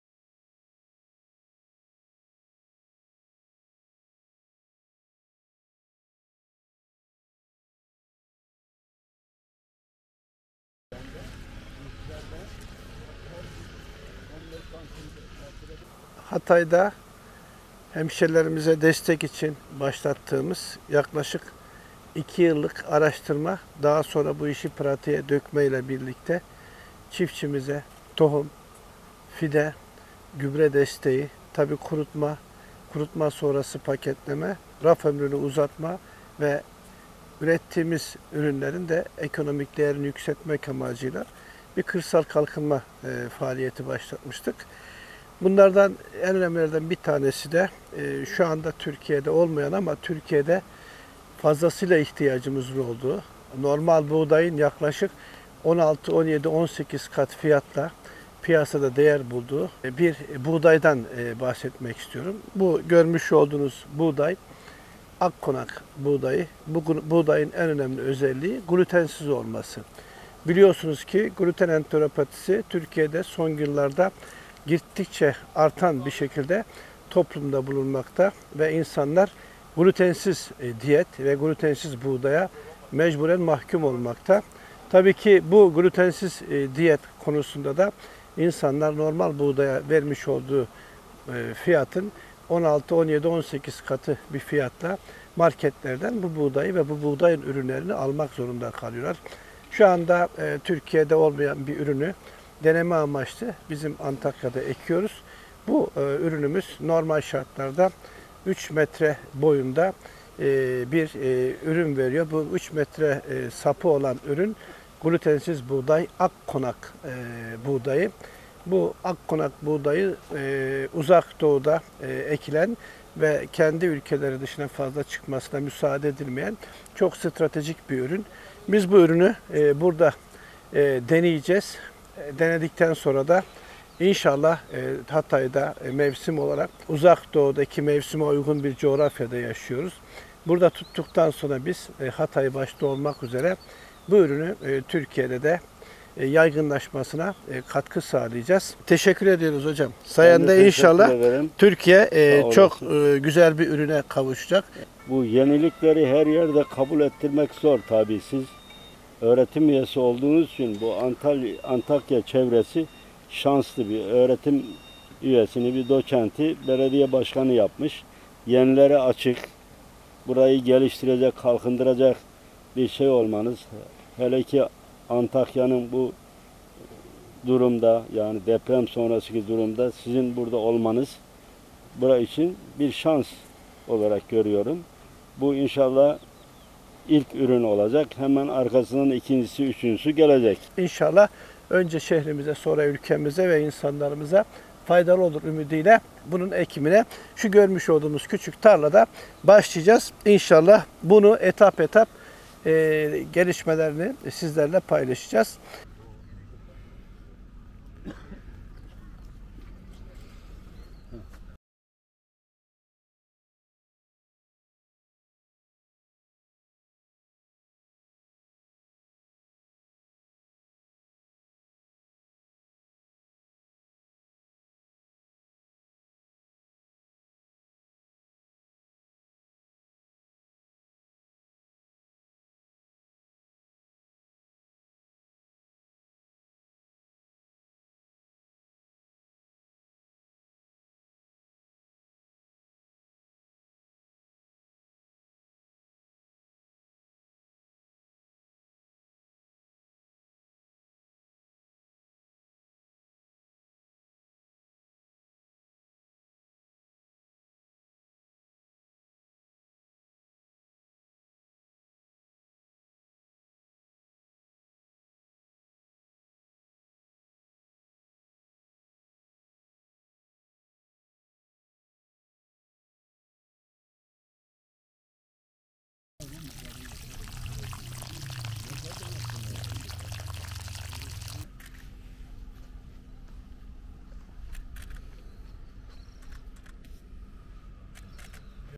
Gelecek nesillerimiz için üretime ve çiftçilerimize destek vermeye devam edeceğiz
AKKONAK BUĞDAYI EXPO MEDENİYETLER BAHÇESİNDE TOPRAKLA BULUŞTU